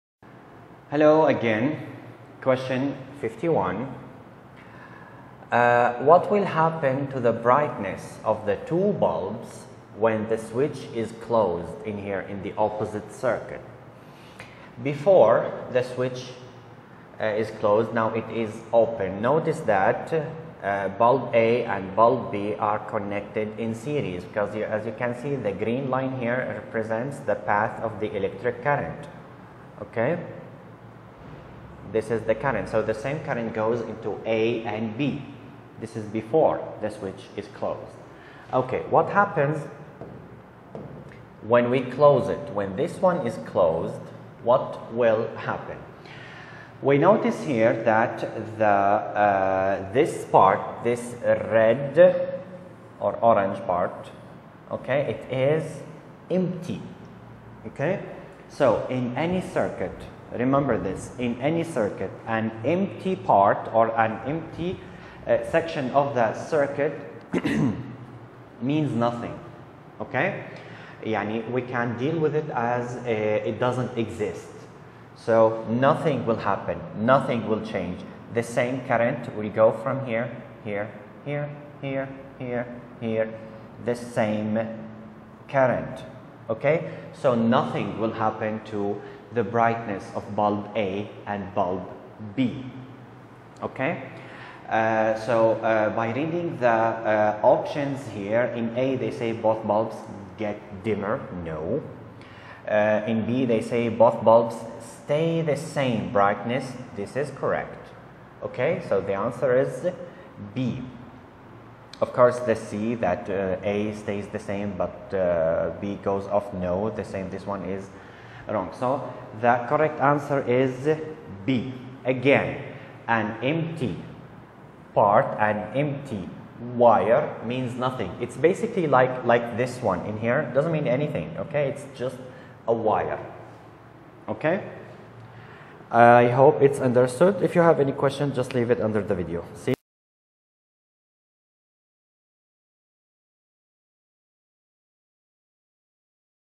51) What will happen to the brightness of the two bulbs when the switch is closed?
51) What will happen to the brightness of the two bulbs when the switch is closed?
A) Both bulbs get dimmer
B) Both bulbs stay the same brightness
C) Bulb A stays the same brightness and bulb B goes out
D) Bulb A stays the same the same and bulb B gets brighter